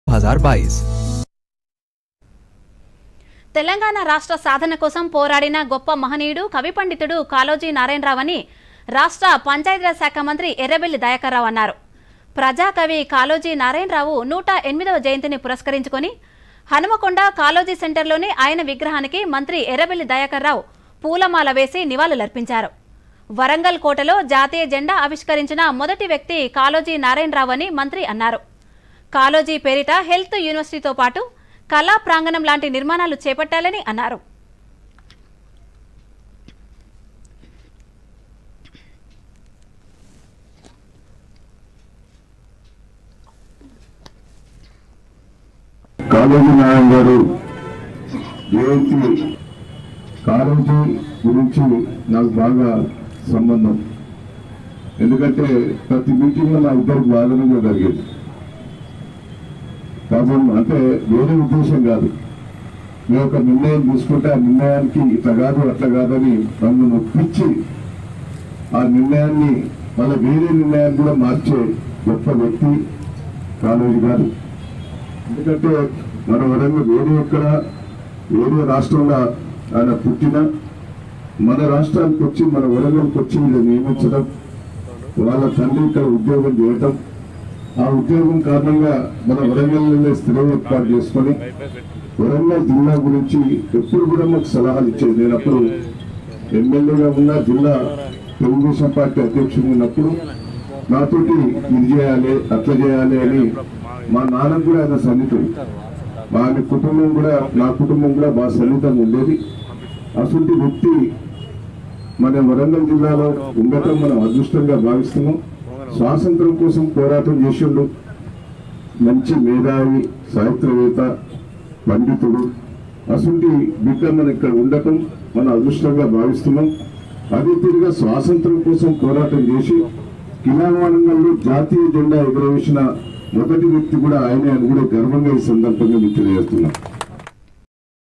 Min Errabelli Dayakar Rao pays tributes to statue of Prajakavi Kaloji | 🟥 DD News Telangana
తెలంగాణ రాష్ట్ర సాధన కోసం పోరాడిన గొప్ప మహనీయుడు, కవి పండితుడు  కాళోజి నారాయణరావు అని రాష్ట్ర పంచాయతీరాజ్ శాఖ మంత్రి ఎర్రబెల్లి దయాకర్ రావు అన్నారు. ప్రజాకవి కాళోజి నారాయణరావు 108వ జయంతిని పురస్కరించుకొని... హనుమకొండ కాళోజి సెంటర్లోని ఆయన విగ్రహానికి మంత్రి ఎర్రబెల్లి దయాకర్ రావు పూలమాలవేసి నివాళులు అర్పించారు. వరంగల్ కోటలో జాతీయ జెండా ఆవిష్కరించిన మొదటి వ్యక్తి  కాళోజి నారాయణరావు అని మంత్రి అన్నారు.   కాళోజి పేరిట హెల్త్ యూనివర్సిటీతో పాటు,  కళా ప్రాంగణం లాంటి నిర్మాణాలు చేపట్టారన్నారు.